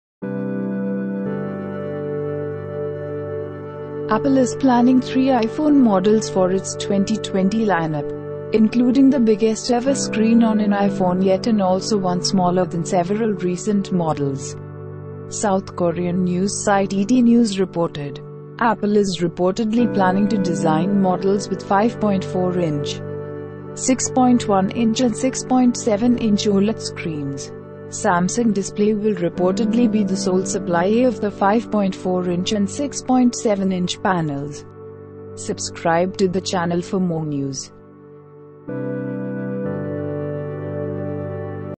Apple may launch biggest screen on an iPhone yet in 2020 lineup
Apple is planning three iPhone models for its 2020 lineup, including the biggest-ever screen on an iPhone yet and also one smaller than several recent models, South Korean news site ETNews reported. Apple is reportedly planning to design models with 5.4-inch, 6.1-inch and 6.7-inch OLED screens. Samsung Display will reportedly be the sole supplier of the 5.4-inch and 6.7-inch panels.